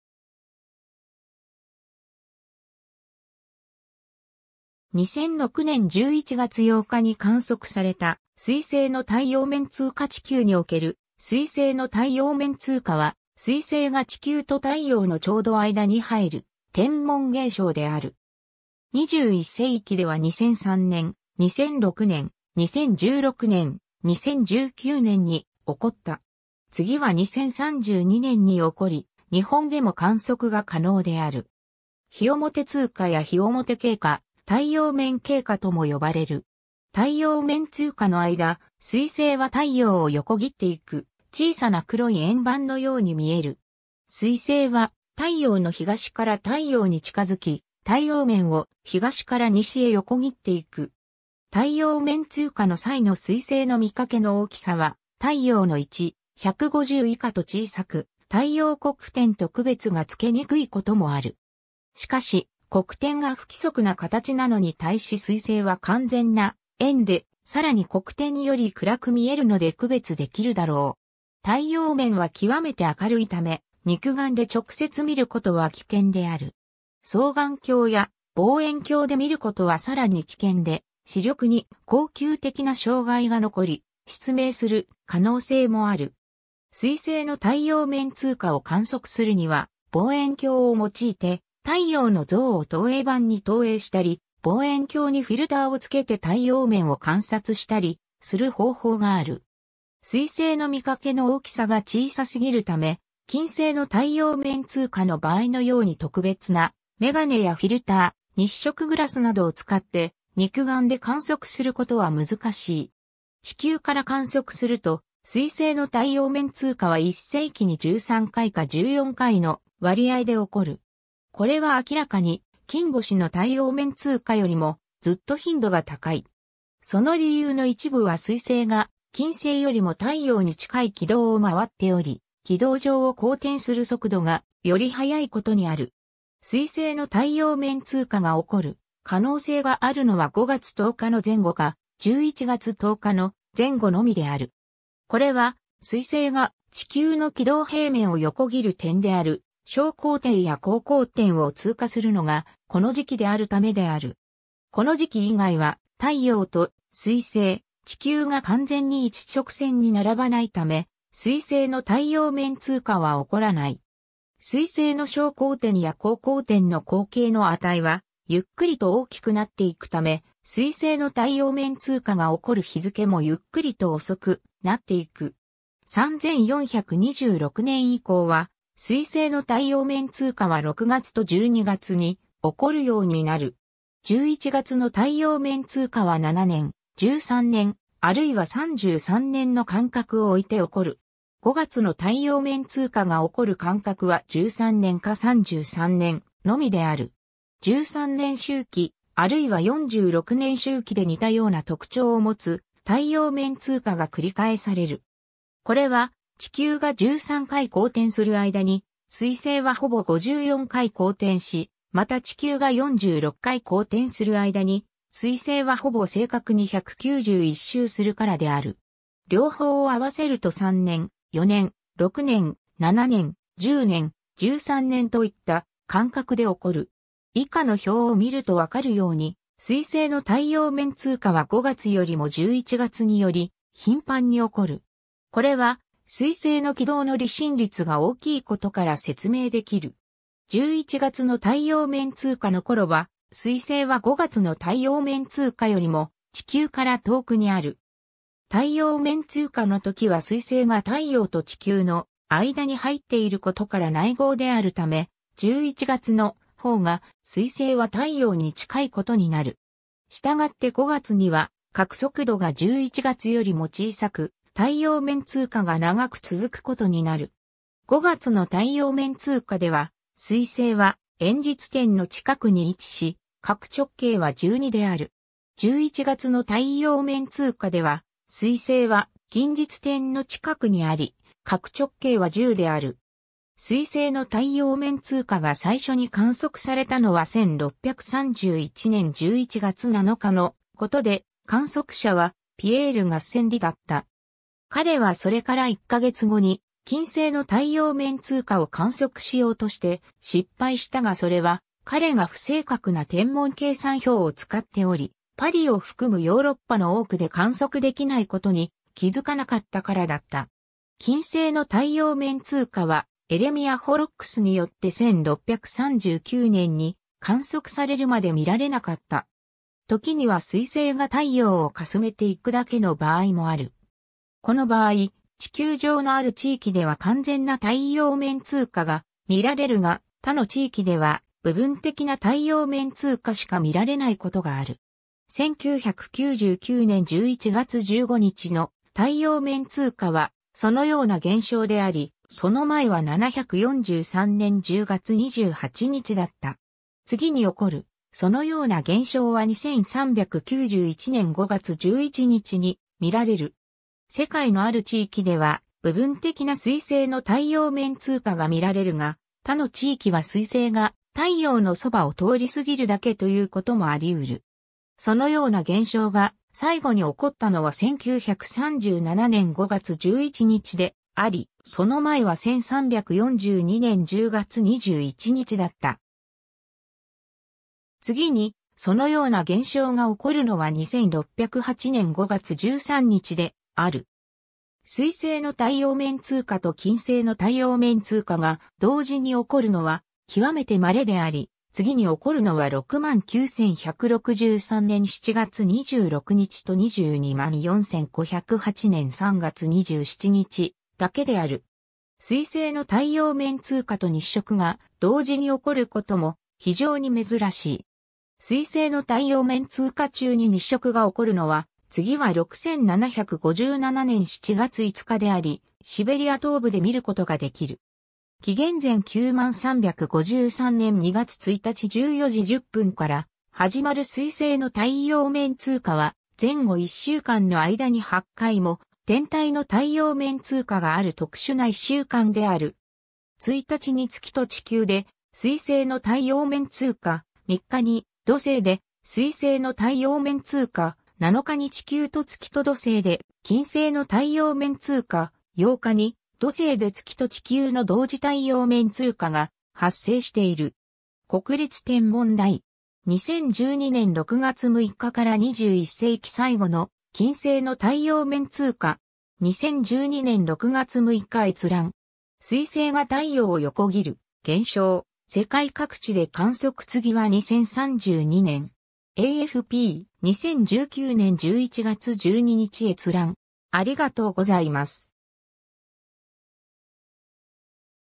水星の太陽面通過
2006年11月8日に観測された水星の太陽面通過 地球における水星の太陽面通過（すいせいのたいようめんつうか）は、水星が地球と太陽のちょうど間に入る天文現象である。
21世紀では2003年、2006年、2016年、2019年に起こった。
次は2032年に起こり、日本でも観測が可能である。
日面通過や日面経過、太陽面経過とも呼ばれる。
 太陽面通過の間、水星は太陽を横切っていく小さな黒い円盤のように見える。
水星は太陽の東から太陽に近づき、太陽面を東から西へ横切っていく。
太陽面通過の際の水星の見かけの大きさは太陽の1/150以下と小さく、太陽黒点と区別が付けにくいこともある。
しかし、黒点が不規則な形なのに対し水星は完全な円でさらに黒点より暗く見えるので区別できるだろう。
 太陽面は極めて明るいため、肉眼で直接見ることは危険である。
双眼鏡や望遠鏡で見ることはさらに危険で視力に恒久的な障害が残り、失明する可能性もある。
水星の太陽面通過を観測するには望遠鏡を用いて太陽の像を投影版に投影したり、望遠鏡にフィルターを付けて太陽面を観察したりする方法がある。
水星の見かけの大きさが小さすぎるため金星の太陽面通過の場合のように特別な眼鏡やフィルター、日食グラスなどを使って肉眼で観測することは難しい。
 地球から観測すると、水星の太陽面通過は1世紀に13回か14回の割合で起こる。
これは明らかに金星の太陽面通過よりもずっと頻度が高い。
その理由の一部は水星が金星よりも太陽に近い軌道を回っており、軌道上を公転する速度がより速いことにある。
 水星の太陽面通過が起こる可能性があるのは5月10日の前後か、11月10日の前後のみである。
これは、水星が地球の軌道平面を横切る点である昇交点や降交点を通過するのがこの時期であるためである。
この時期以外は太陽と水星、地球が完全に一直線に並ばないため水星の太陽面通過は起こらない。
水星の昇交点や降交点の黄経の値はゆっくりと大きくなっていくため、水星の太陽面通過が起こる日付もゆっくりと遅くなっていく。
3426年以降は、水星の太陽面通過は6月と12月に起こるようになる。
 11月の太陽面通過は7年、13年、あるいは33年の間隔を置いて起こる。
5月の太陽面通過が起こる間隔は13年か33年のみである。
13年周期、あるいは46年周期で似たような特徴を持つ太陽面通過が繰り返される。
これは地球が13回公転する間に水星はほぼ54回公転し、また地球が46回公転する間に水星はほぼ正確に191周するからである。
両方を合わせると3年、4年、6年、7年、10年、13年といった間隔で起こる。
 以下の表を見ると分かるように、水星の太陽面通過は5月よりも11月により頻繁に起こる。
これは水星の軌道の離心率が大きいことから説明できる。
11月の太陽面通過の頃は、水星は5月の太陽面通過よりも地球から遠くにある。
太陽面通過の時は水星が太陽と地球の間に入っていることから内合であるため、11月のほうが水星は太陽に近いことになる。
従って5月には角速度が11月よりも小さく、太陽面通過が長く続くことになる。
 5月の太陽面通過では水星は遠日点の近くに位置し、角直径は12″である。
11月の太陽面通過では水星は近日点の近くにあり、角直径は10″である。
 水星の太陽面通過が最初に観測されたのは1631年11月7日のことで、観測者はピエール・ガッセンディだった。
彼はそれから1ヶ月後に金星の太陽面通過を観測しようとして失敗したがそれは彼が不正確な天文計算表を使っており、パリを含むヨーロッパの多くで観測できないことに気付かなかったからだった。
金星の太陽面通過はエレミア・ホロックスによって1639年に観測されるまで見られなかった。
 時には水星が太陽をかすめていくだけの場合もある。
この場合、地球上のある地域では完全な太陽面通過が見られるが、他の地域では部分的な太陽面通過（第2接触と第3接触が無い）しか見られないことがある。
1999年11月15日の太陽面通過はそのような現象であり、その前は743年10月28日だった。
次に起こるそのような現象は2391年5月11日に見られる。
 世界のある地域では部分的な水星の太陽面通過が見られるが、他の地域は水星が太陽のそばを通り過ぎるだけということも有り得る。
そのような現象が最後に起こったのは1937年5月11日であり、その前は1342年10月21日だった。
次にそのような現象が起こるのは26...